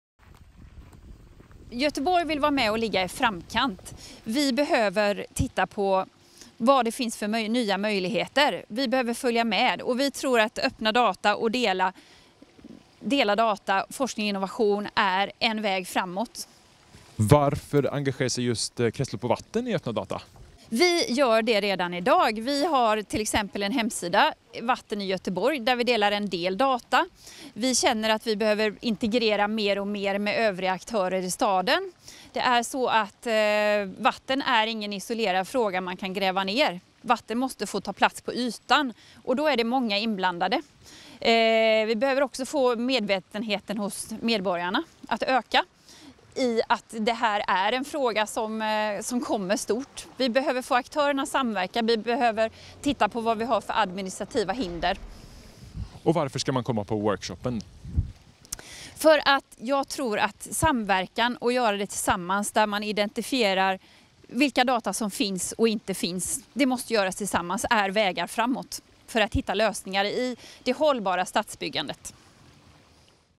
Lena Blom om öppna data i Göteborgs stad
Lena Blom från Kretslopp och vatten i Göteborgs stad intervjuas om vikten av öppna data som framgångsfaktor för ökad innovation.